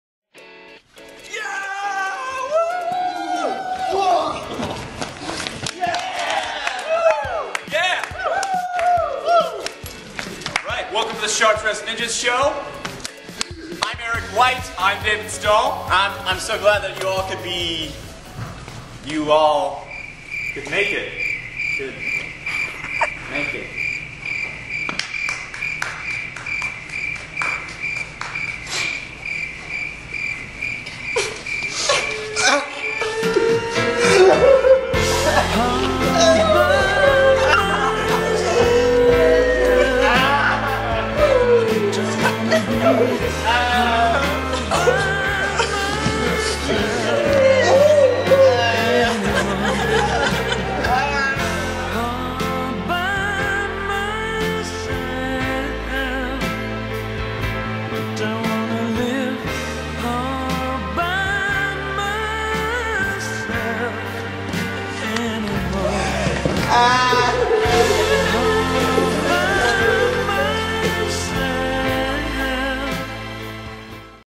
See The Sharp-Dressed Ninjas on February 8th
You could be in that audience! Come to The Sharp-Dressed Ninjas' improv show at 7pm on February 8th. It's free.